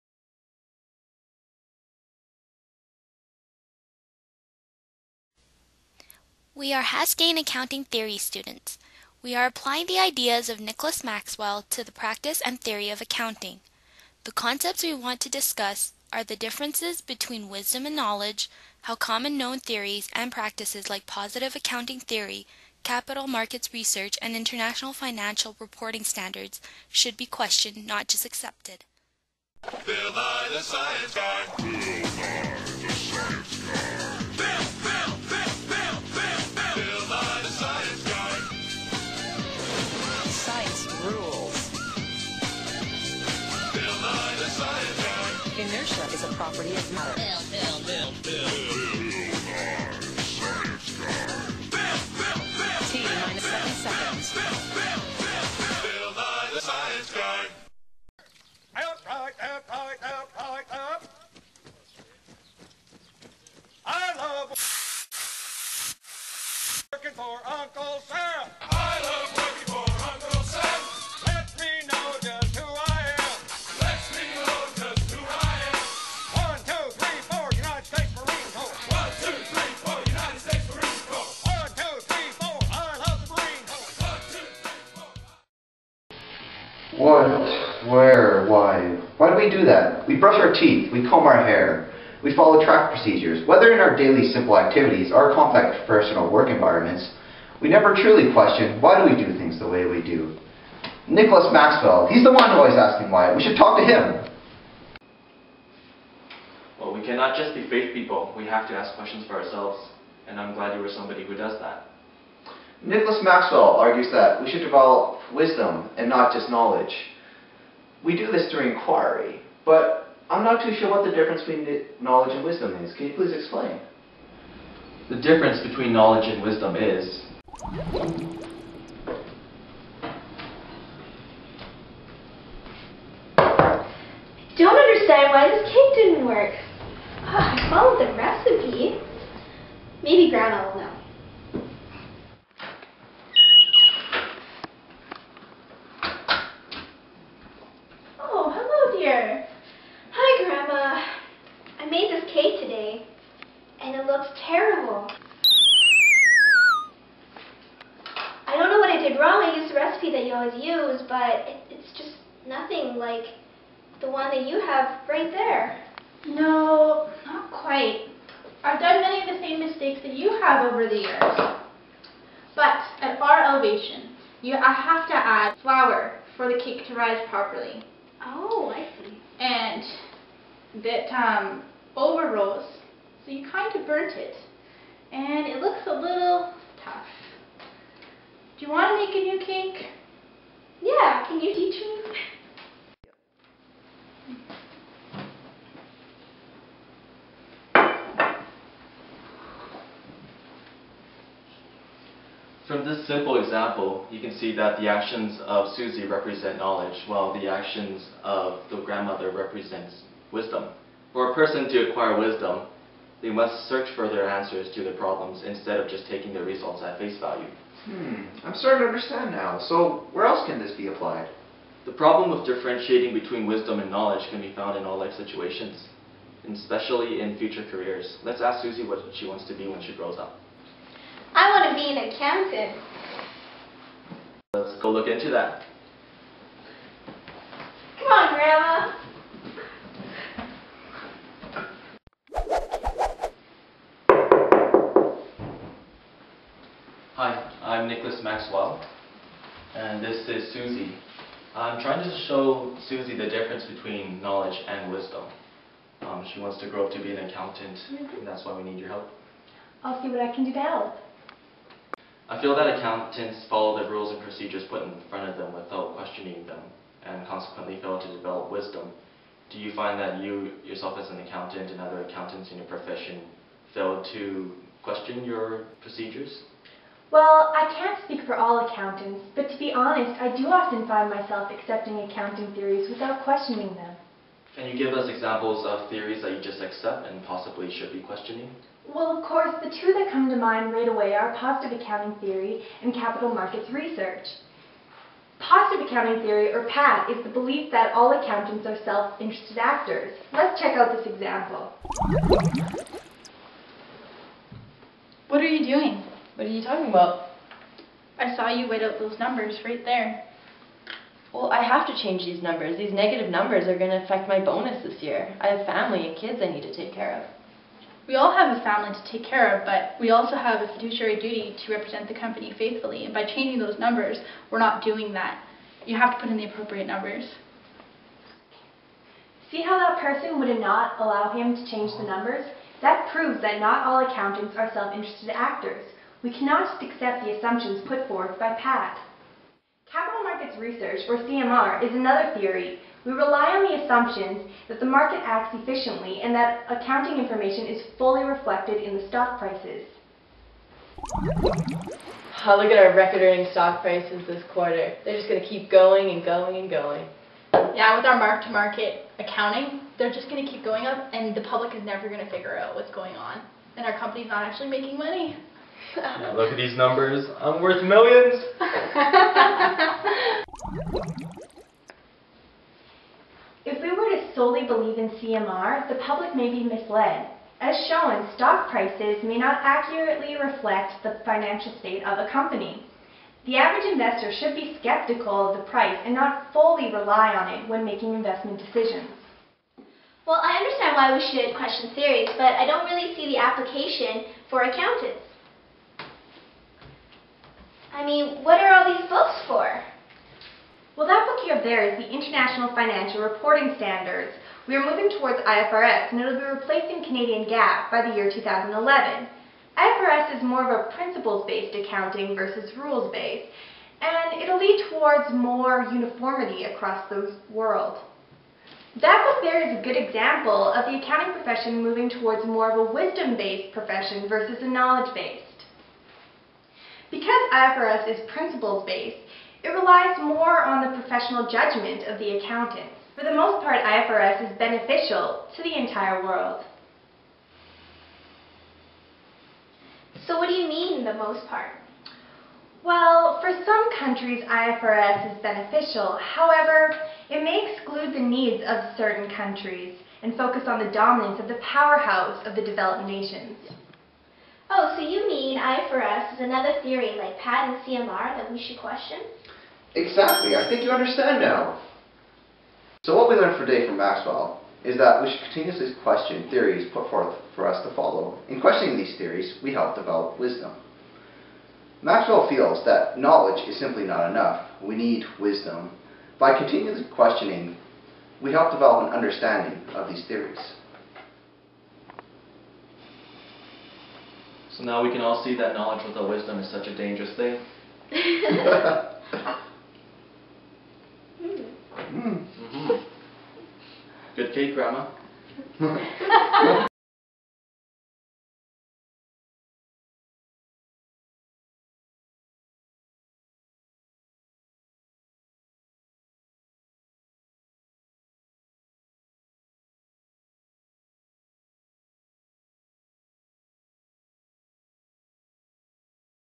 UofC Accounting Theory and the Ideas of Nicholas Maxwell (Group Three)
A project video for ACCT 445 at the University of Calgary.  We explore the ideas of Nicholas Maxwell (CBC ``How to think about Science``) and how they apply to accounting theory.